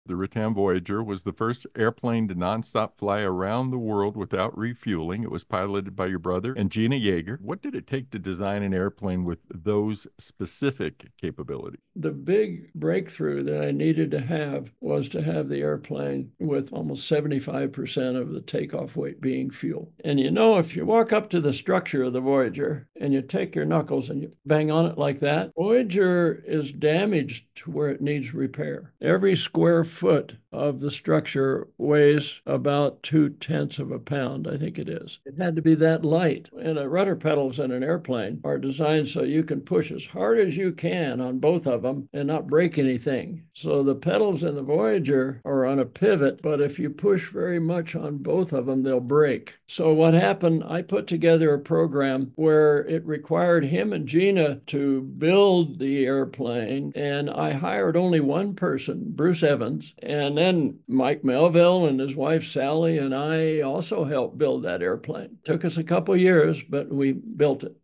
Designing the Record-Breaking Rutan Voyager
Aerospace Engineer Burt Rutan discusses building the first aircraft to fly around the world without refueling and the challenges he faced along the way.

To hear more, check out episode 50 of the Behind the Wings podcast wherever you listen!